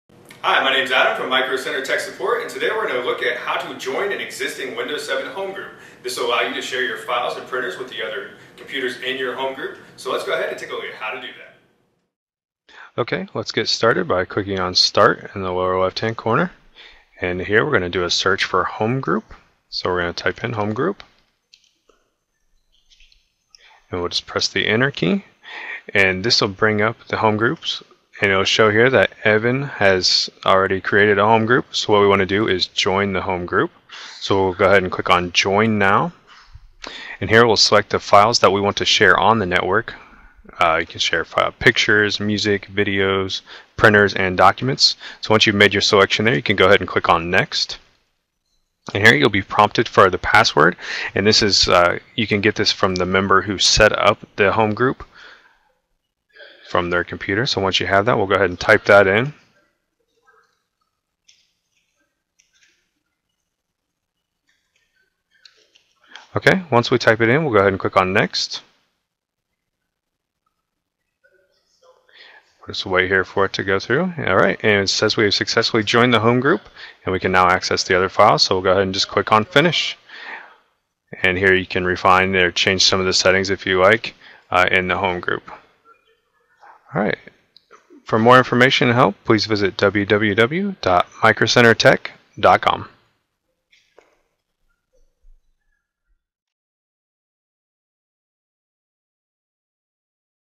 Tech Support: How to Join a Windows 7 HomeGroup
Micro Center Tech Support takes you through the steps of how to join an existing HomeGroup setup on a local area network.